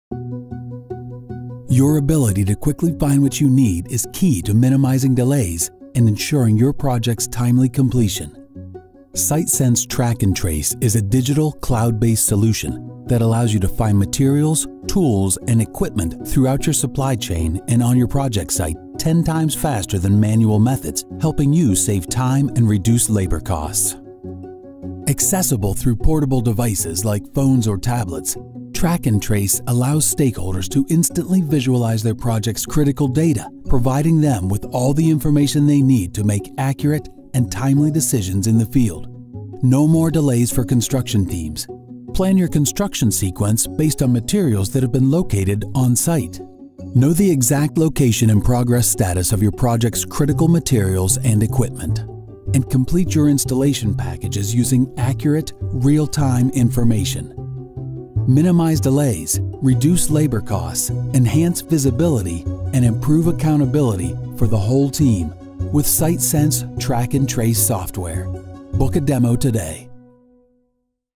SiteSense Track and Trace Overview
Intelliwave Technologies' SiteSense® Track and Trace is a Cloud-based web and mobile construction SaaS solution that utilizes IoT Sensors such as RFID, BLE and GPS to track the near real-time location of critical path materials, tools, assets and equipment for construction projects. Workflow and progress tracking for all phases of construction projects from fabrication to installation can be tracked using this SiteSense module.